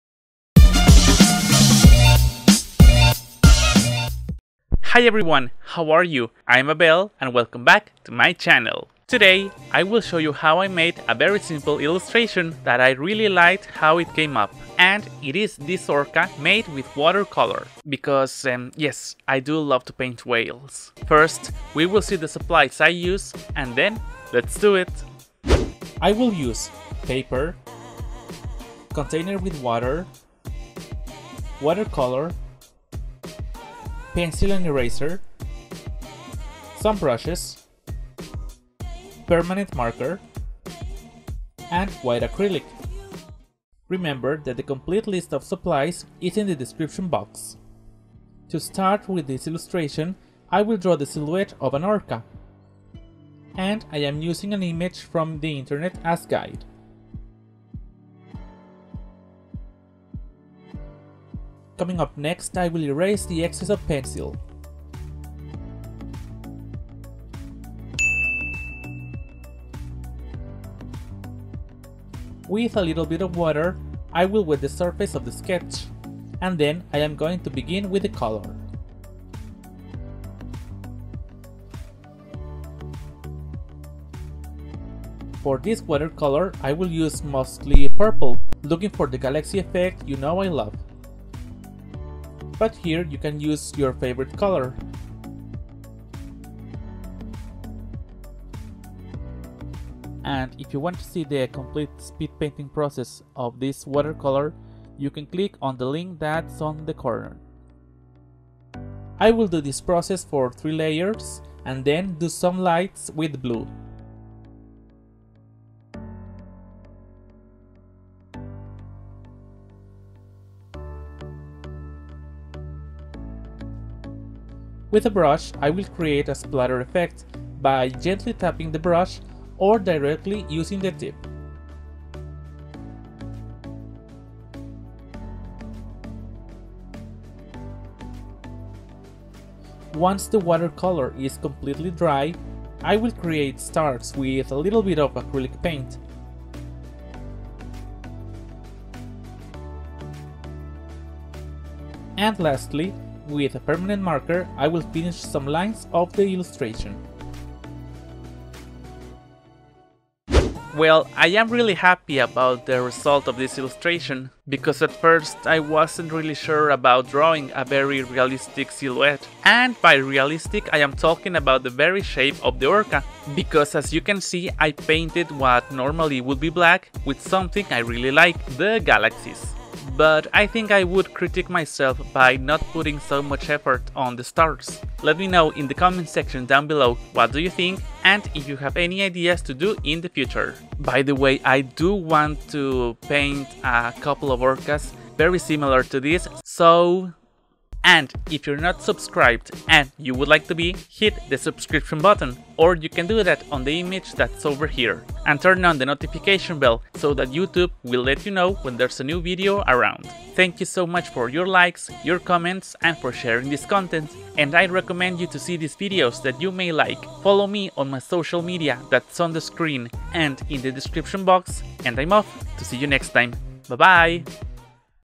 Make This Tumblr Orca With Watercolor! 🐳🎨 - DIY [Abel Rosales]
Hi there! Thanks for reading the description! For today's video I will combine two of my favorite thing s to do: whales and galaxies. Hope you try it and enjoy it!

I used:

Paper
Container with water
Watercolor
Pencil and eraser
Some brushes
Permanent marker
White acrylic